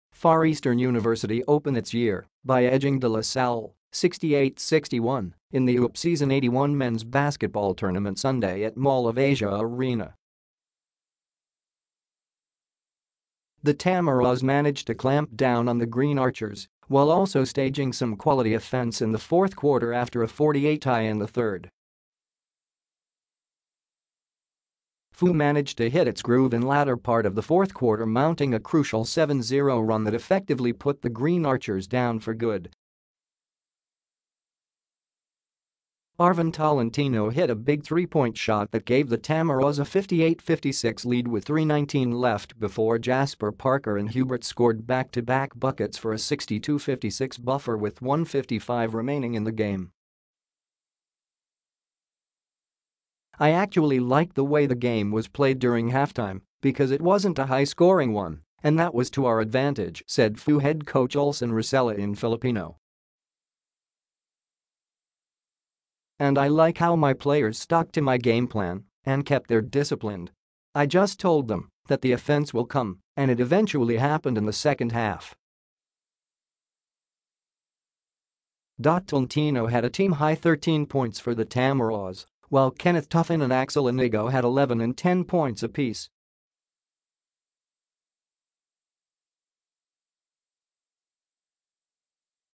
FEU clips La Salle to open UAAP Season 81 campaign
Far Eastern University opened its year by edging De La Salle, 68-61, in the UAAP Season 81 men’s basketball tournament Sunday at Mall of Asia Arena.

The Tamaraws managed to clamp down on the Green Archers while also staging some quality offense in the fourth quarter after a 48-tie in the third.

FEU managed to hit its groove in latter part of the fourth quarter mounting a crucial 7-0 run that effectively put the Green Archers down for good.

Arvin Tolentino hit a big 3-point shot that gave the Tamaraws a 58-56 lead with 3:19 left before Jasper Parker and Hubert scored back-to-back buckets for a 62-56 buffer with 1:55 remaining in the game.

“I actually liked the way the game was played during halftime because it wasn’t a high scoring one and that was to our advantage,” said FEU head coach Olsen Racela in Filipino.

“And I like how my players stuck to my game plan and kept their discipline. I just told them that the offense will come and it eventually happened in the second half.”

Toelntino had a team-high 13 points for the Tamaraws while Kenneth Tuffin and Axel Iñigo had 11 and 10 points apiece.

Santi Santillan led La Salle with 19 points and 10 boards.